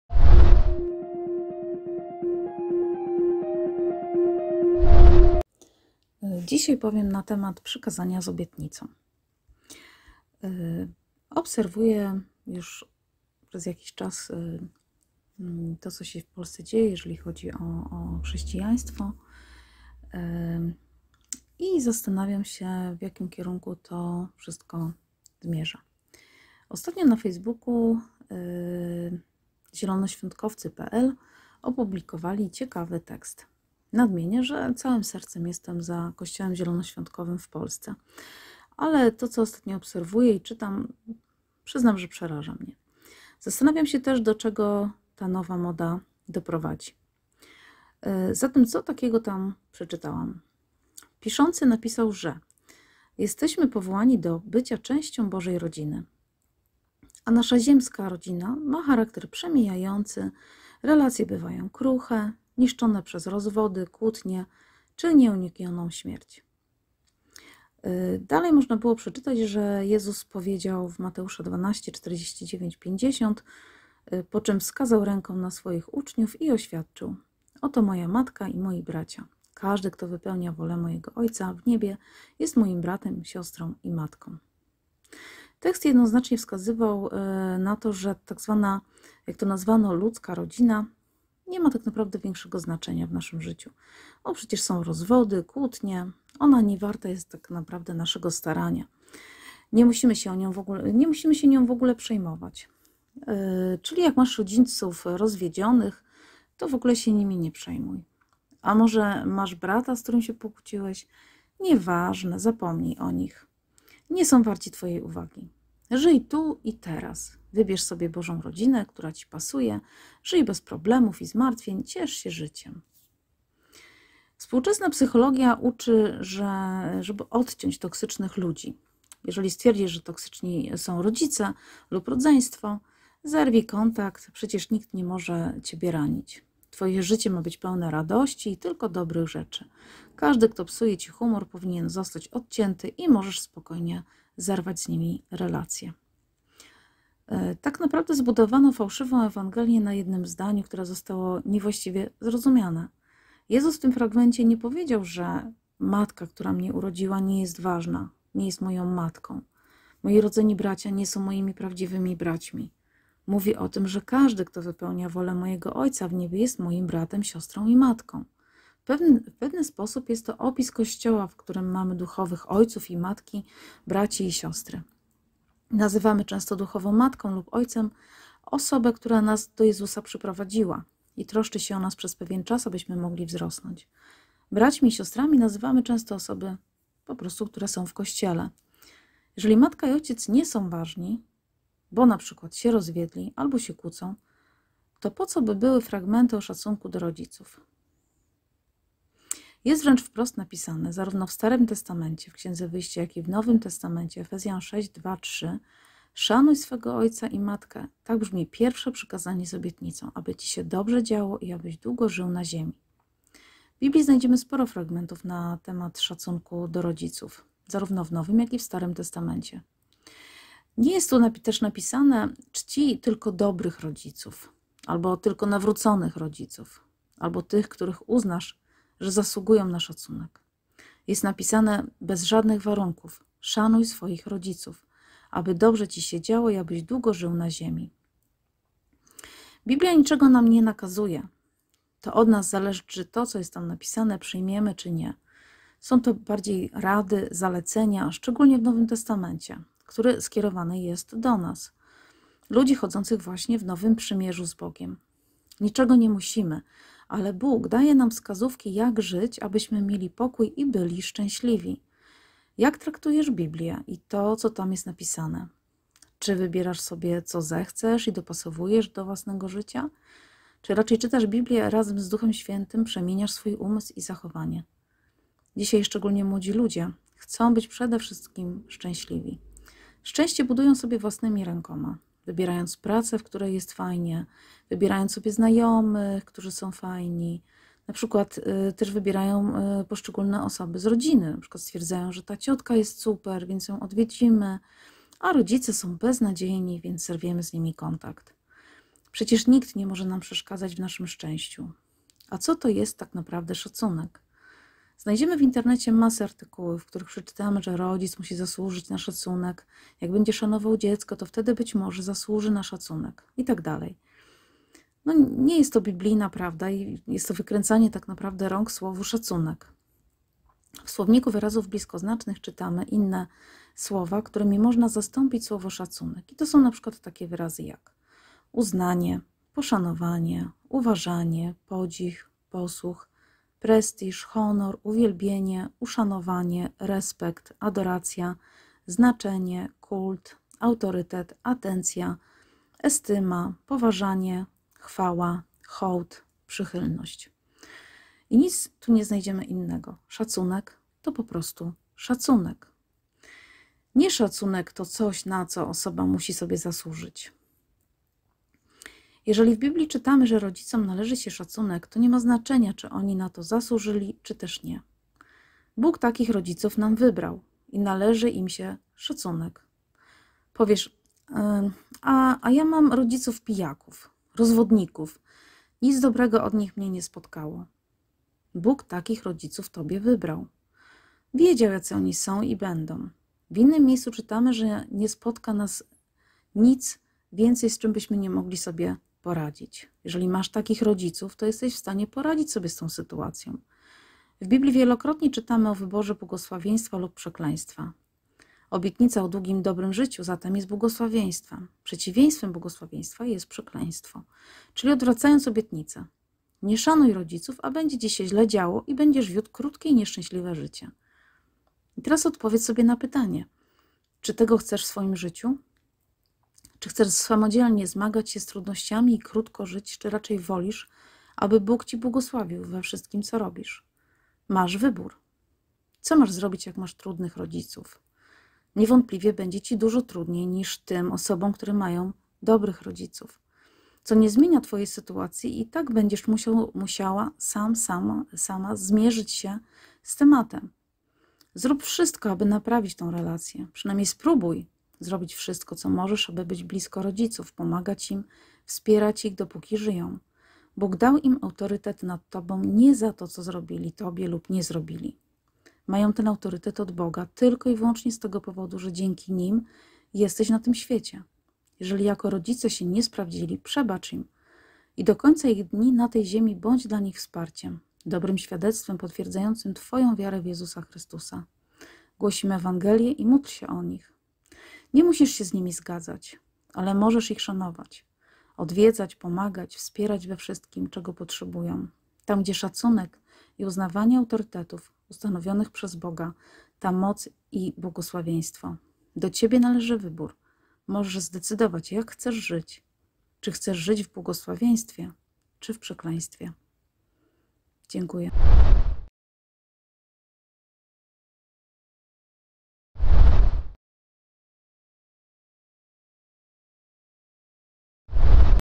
Szanuj rodziców, aby...
Szanuj rodziców, aby... Niekiedy biblijne błogosławieństwa są warunkowe, a przez spłyconą i niepełne rozumienie przekazu ewangelii sami sobie robimy krzywdę. Niestety :-(
Moje książki Kupując je wspierasz rozwój tego kanału.